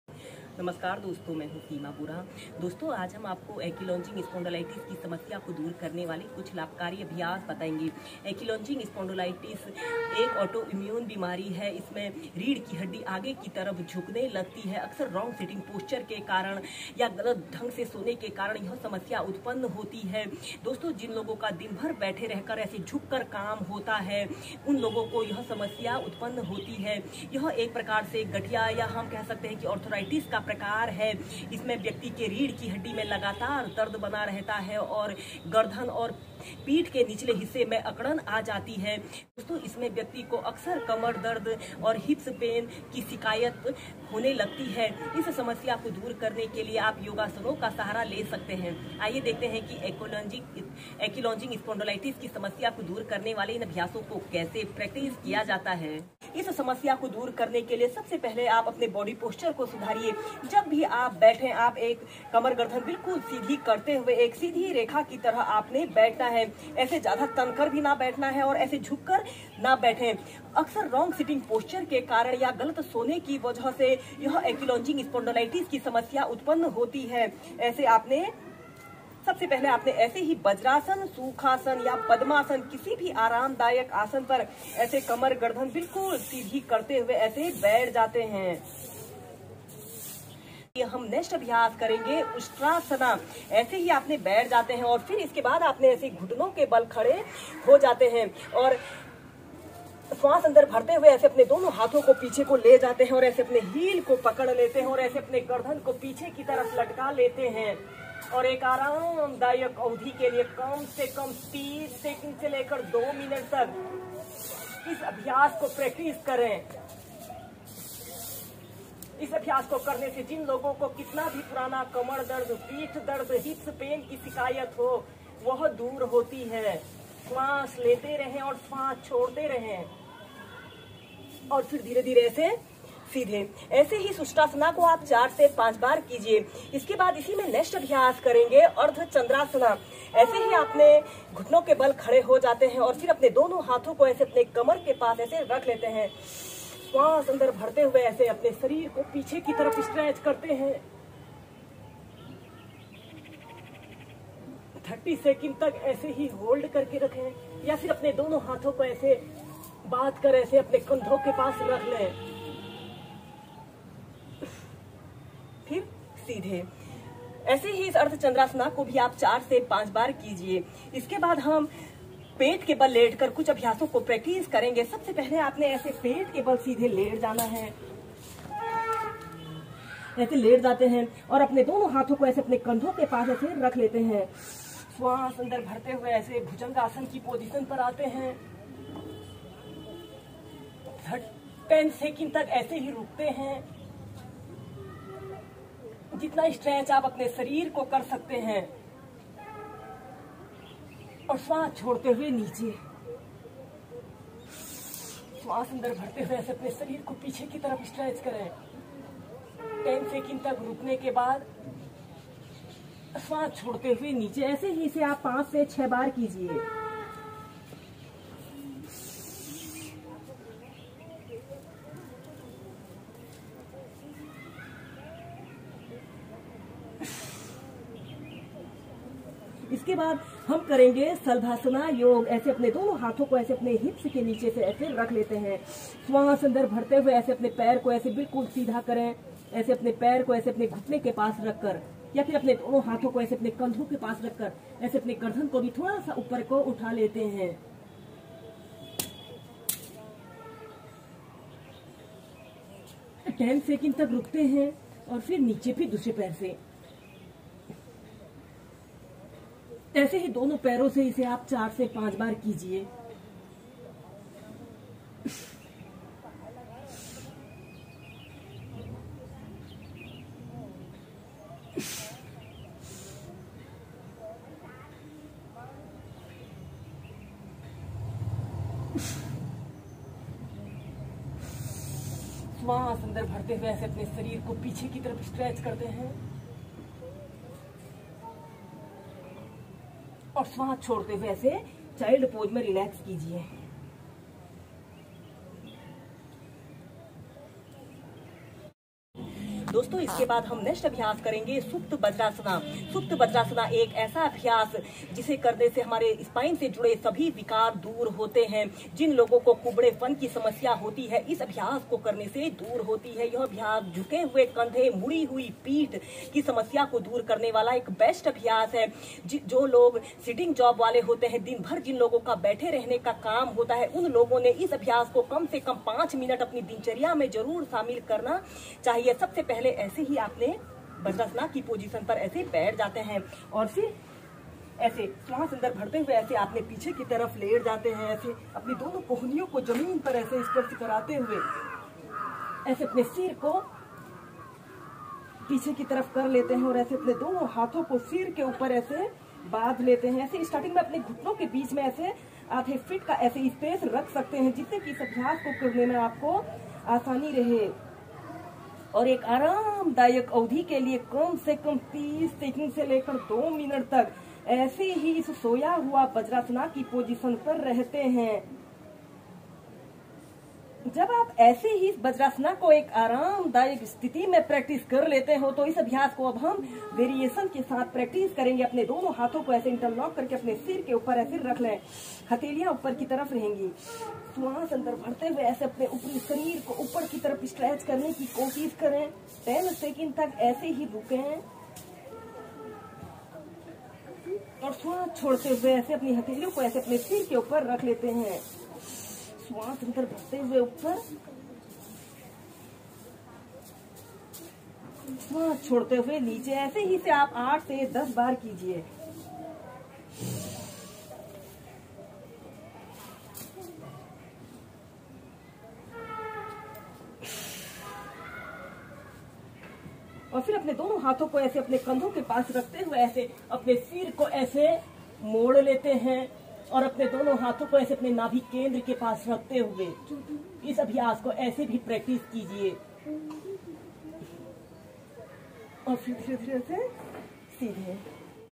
YogaForAnkylosingSpondylitis/पीठ दर्द,लोअर बैकपेन,हिप्स दर्द से निजात पाये/by seema bora
दोस्तों आज हम आपको एंकिलॉजिंग स्पॉन्डिलाइटिस की समस्या को दूर करने वाले कुछ लाभकारी योगासन बता रहे है।(Ankylosing Spondylitis) एक ऑटोइम्यून बीमारी है। इस रोग में रीढ़ की हड्डी आगे की ओर झुकने लगती हैं। खराब बैठने की आदत के कारण यह समस्या सबसे ज्यादा होती है।  इस रोग के शिकार सबसे ज्यादा युवा होते हैं।एंकिलोज़िंग स्पॉन्डिलाइटिस आर्थराइटिस (गठिया) का ही एक प्रकार है। इसमें रीड, कूल्हे की हड्डियों और रीढ़ की जॉइंट्स में बहुत अधिक दर्द हो होता है। इस बीमारी की वजह से रीढ़ की हड्डी का लचीलापन कम हो जाता है और पीड़ित व्यक्ति न ठीक से बैठ पाता है और उसे घूमने फिरने में भी तकलीफ होने लगती है। आप योगासनों के नियमित अभ्यास से इस समस्या को दूर कर सकते है और फिट और हेल्दी लाइफ जी सकते है।by seema bora